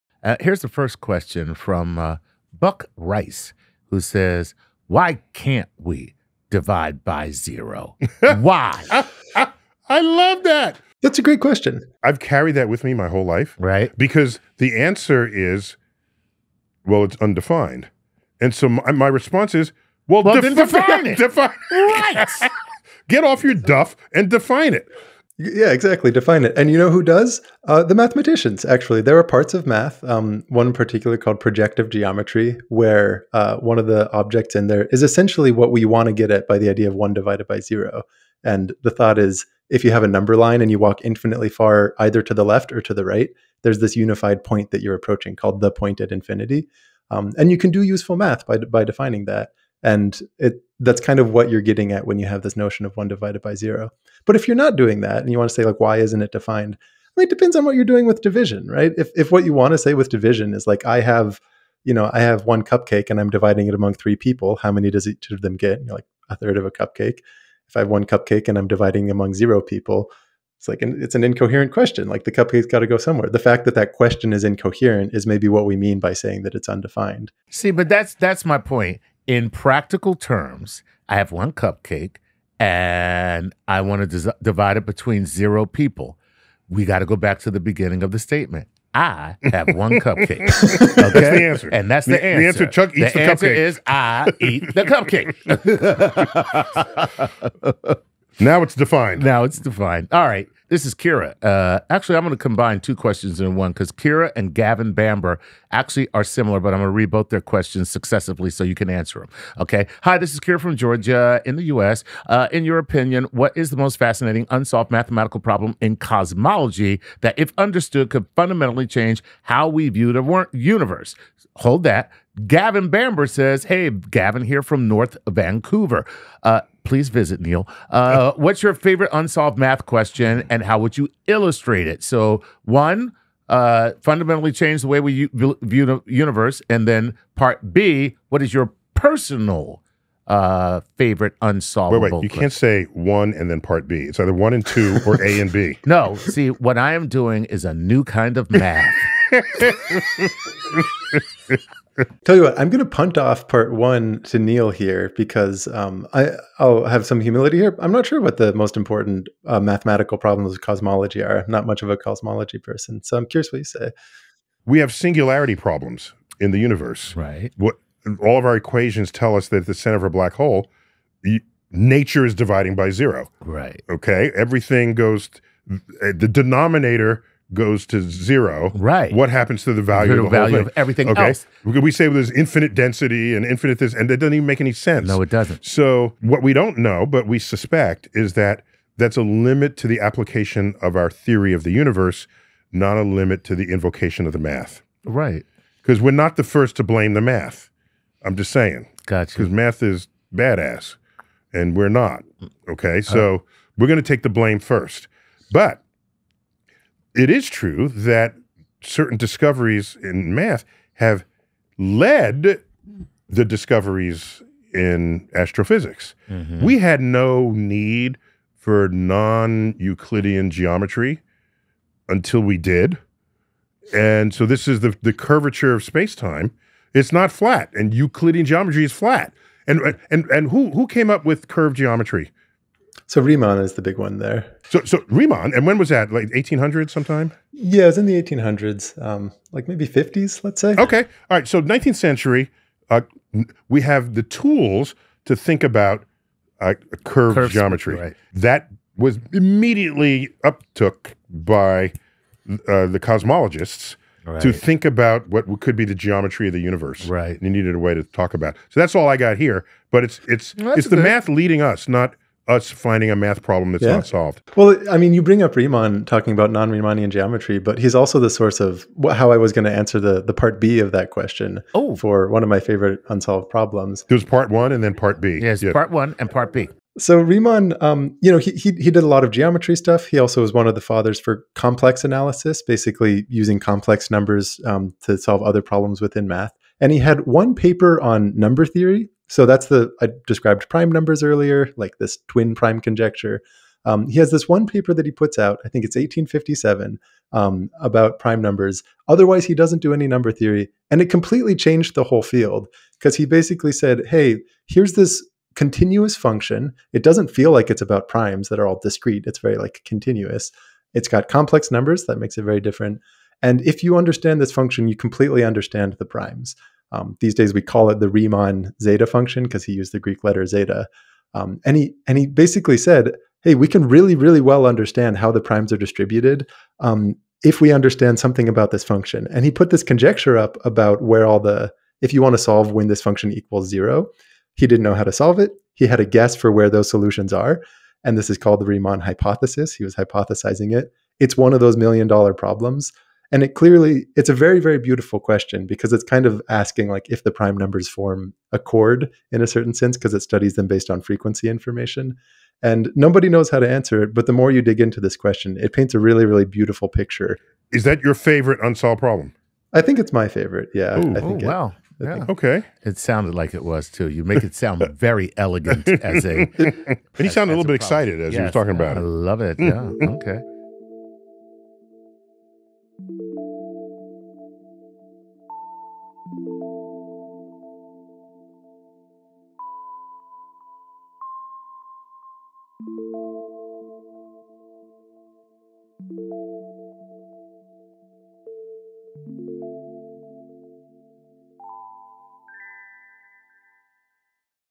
Why We Can’t Divide By Zero
Neil deGrasse Tyson & Grant Sanderson (3Blue1Brown) | Divide by Zero, Black Holes & the Riemann Hypothesis

Why is 1 ÷ 0 “undefined”—and is there any math where it does make sense? Grant shows how projective geometry adds a point at infinity to model the idea, while Neil connects “division by zero” to black-hole singularities—places where equations blow up, signaling the limits of our theory, not bad math. They highlight how new math leads physics (Riemannian geometry → curved spacetime) and end on the Riemann Hypothesis, the $1M problem tying complex analysis to the pattern of primes.